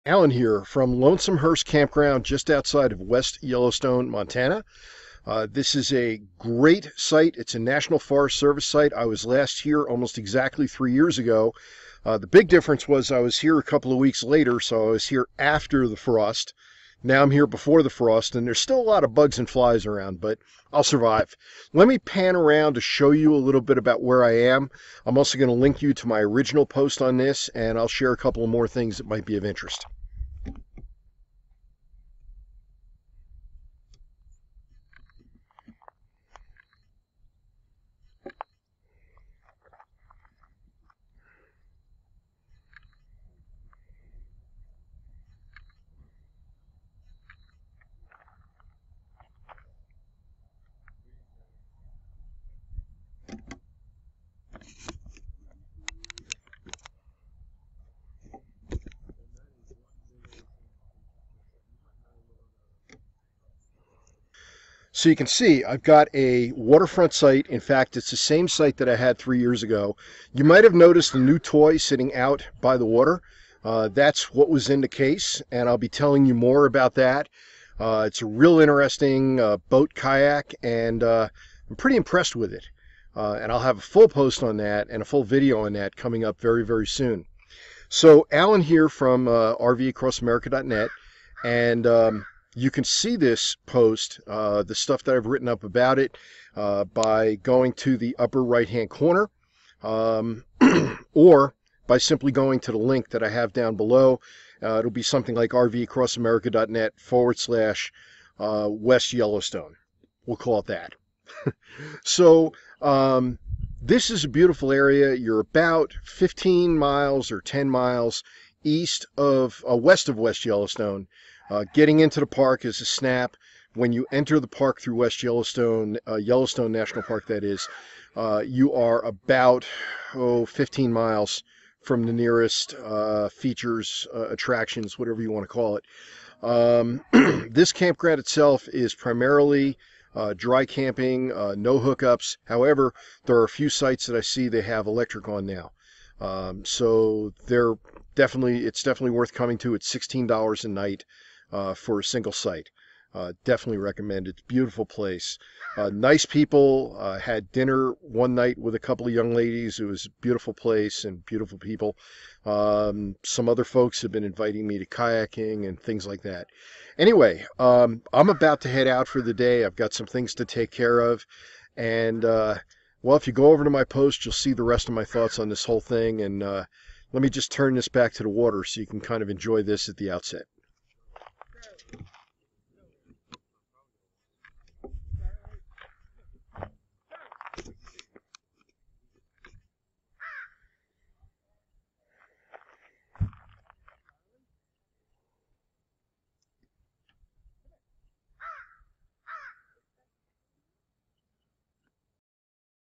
Lonesomehurst - a GREAT National Park Service Campground
Lonesomehurst - simply put - good enough for a return visit! I was here in 2015 and now I'm back. Another 3 nights waterfront - what's not to love?

And I had the opportunity to test out my new kayak - details are within my post - along with another video.